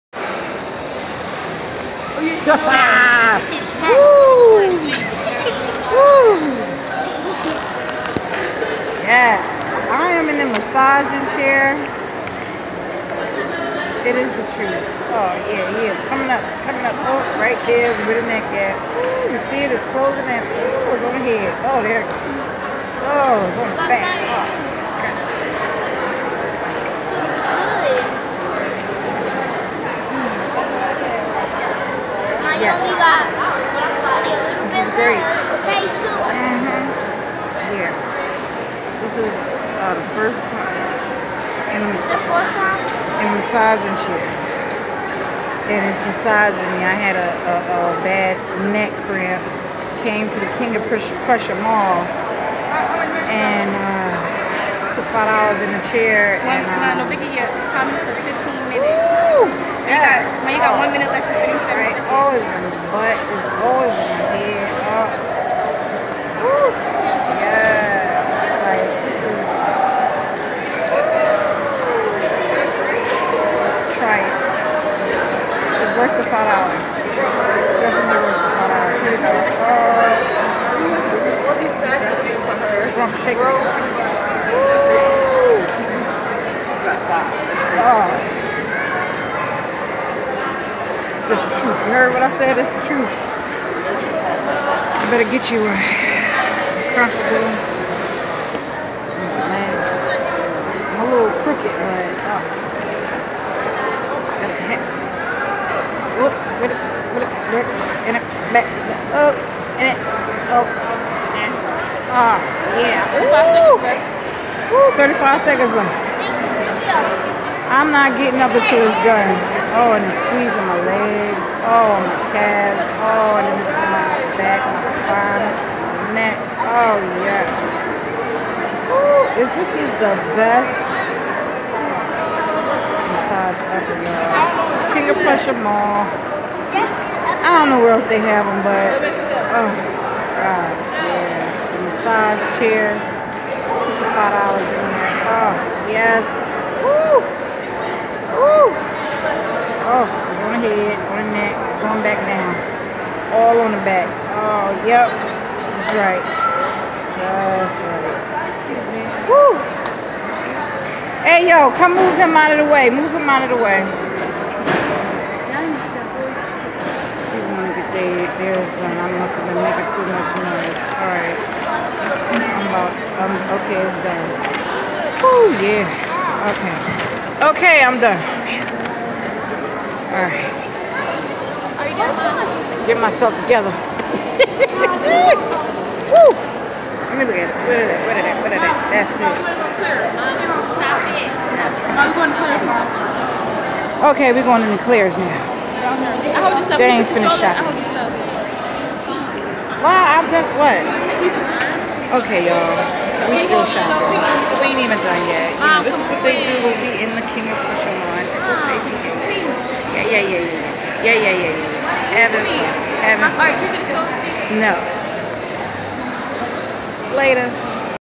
Massage Chair in the KP mall is the truth
Enjoying the massage chair